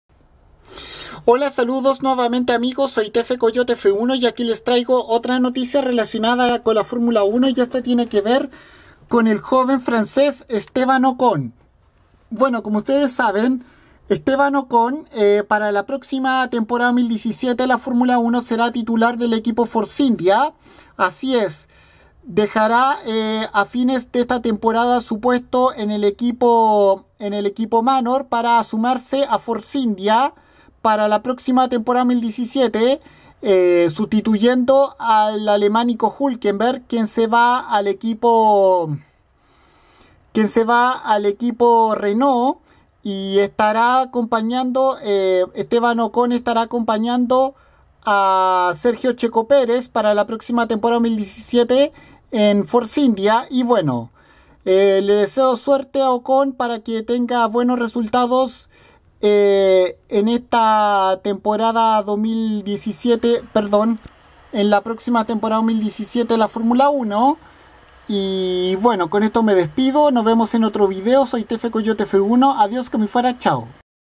Esteban Ocon será titular de Force India en 2017
Para la temporada 2017 de la F1 el equipo Force India ha confirmado al joven francés Esteban Ocon siendo compañero de equipo del mexicano Sergio "Checo" Pérez